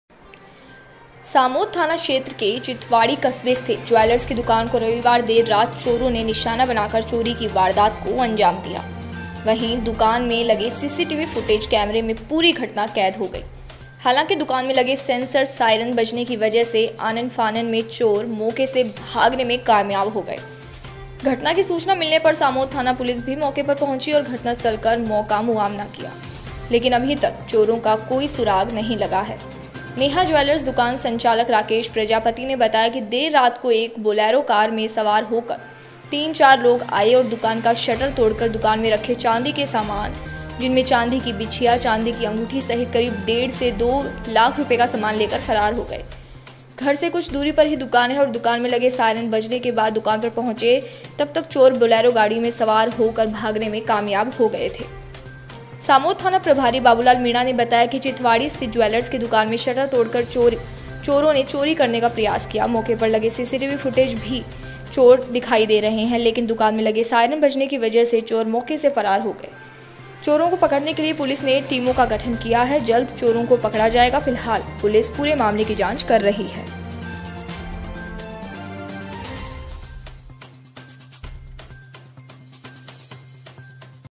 Chomu Jaipur: चोमू से बड़ी खबर!सामोद में ज्वेलर्स की दुकान को चोरों ने बनाया निशाना, दो लाख के करीब
Chomu Jaipur: चोमू से बड़ी खबर!सामोद में ज्वेलर्स की दुकान को चोरों ने बनाया निशाना, दो लाख के करीब माल चोरी